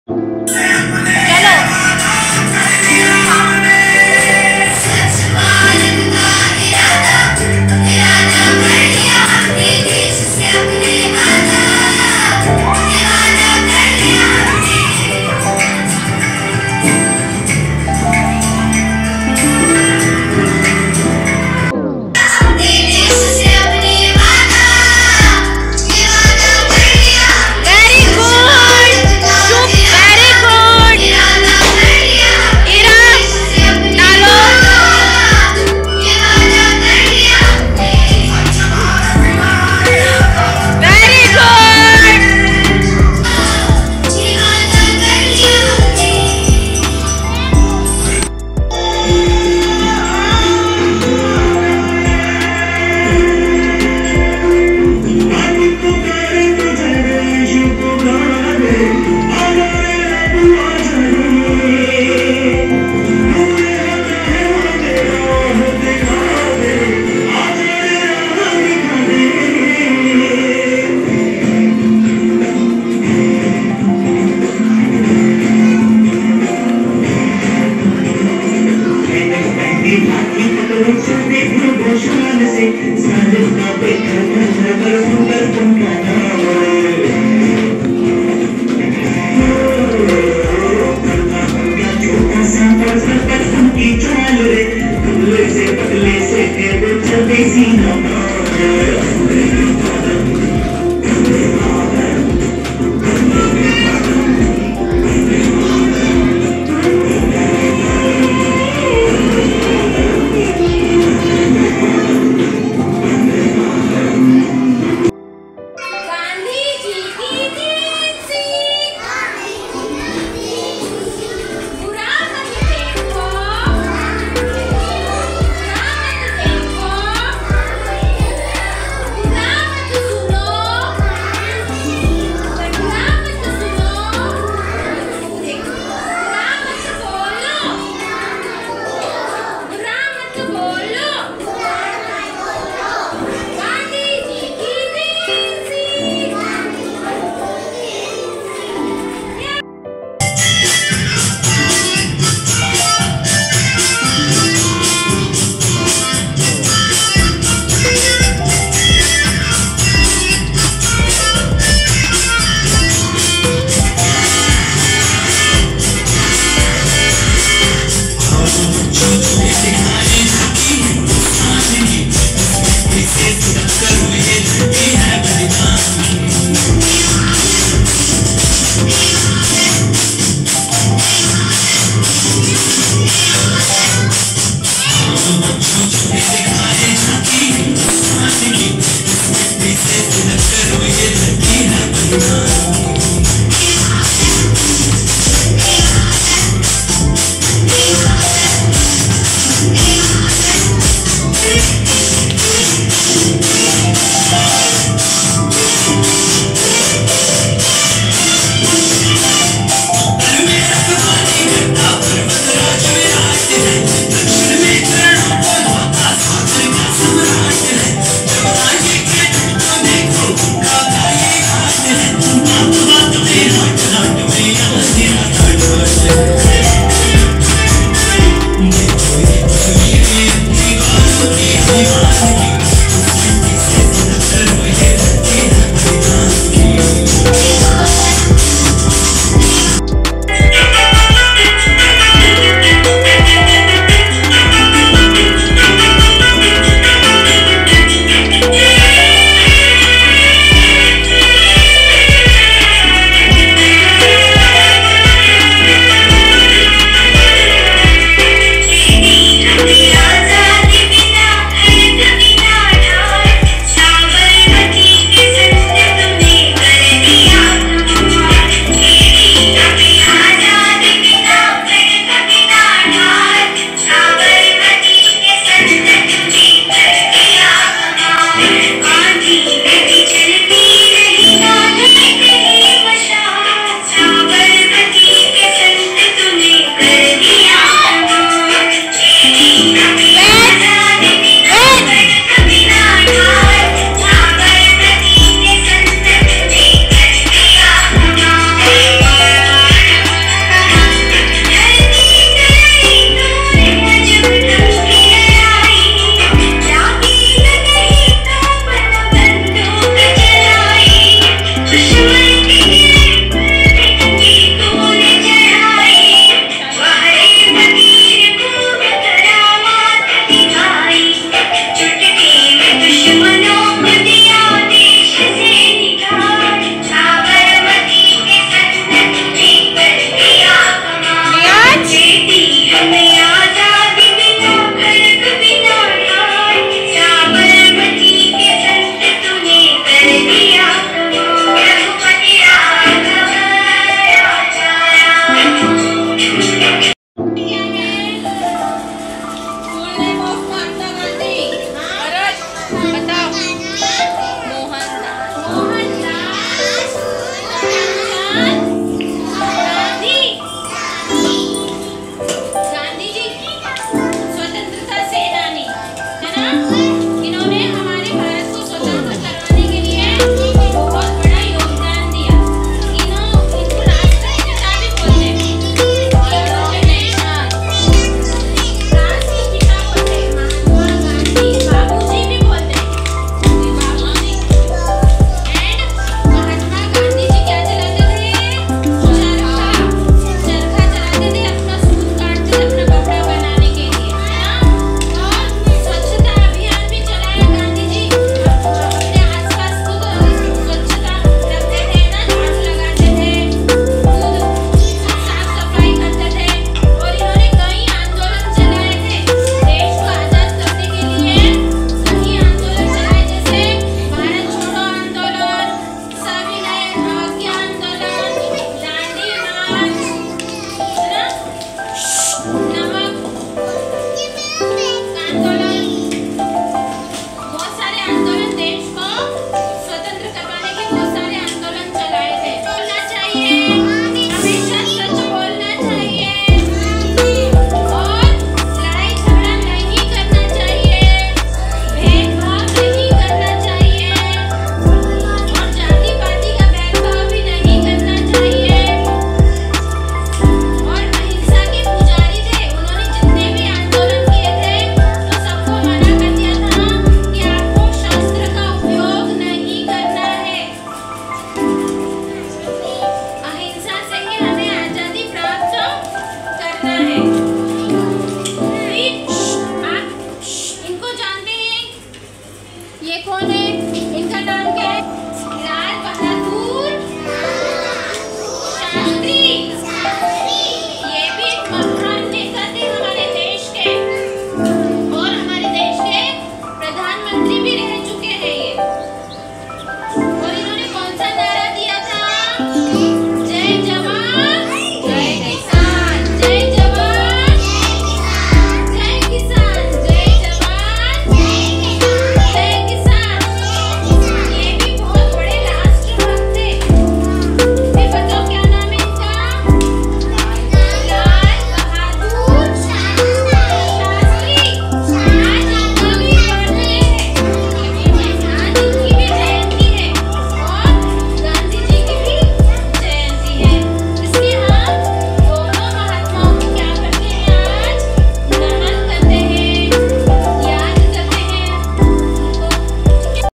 Gandhi Jayanti 2023
Gandhi Jayanti Celebration

Celebration of Gandhi and Lal Bhadur Shastri Jayanti with their teachings .